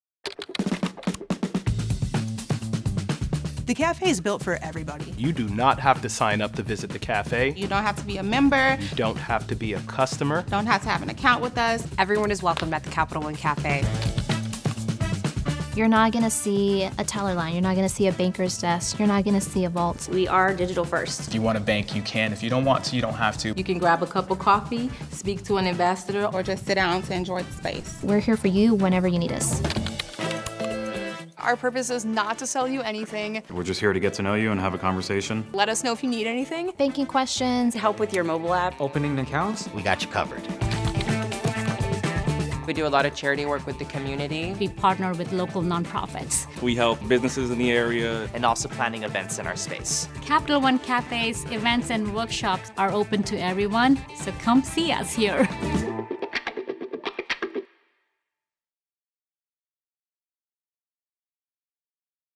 Is Everyone Welcome at Capital One Cafés? | Capital One Café
Everyone is welcome at Capital One Cafés! You don’t need to bank with Capital One to come in and enjoy all we have to offer, because our Cafés are much more than just a bank! Speak to a Café Ambassador, work or study in our cozy spaces with free Wi-Fi access, or simply kick back with a delicious handcrafted beverage. Hear more from our Café Ambassadors about what’s waiting for everyone at a Capital One Café and stop in to experience it for yourself!